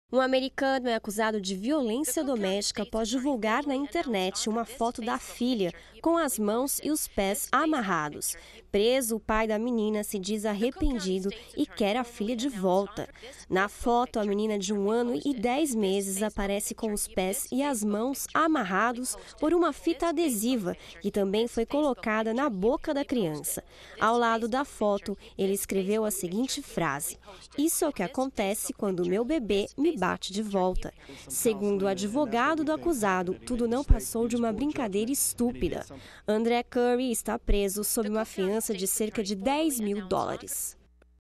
Pai é preso após fotografar filha com mãos e pés amarrados
Após prender membros e boca da criança com fita adesiva, americano divulgou imagem na internet;